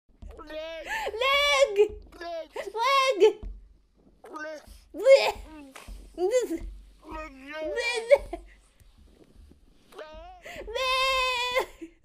l'egg seal - but a Snail!
Clip from Twitch where I found out my spirit animal is the l'egg seal!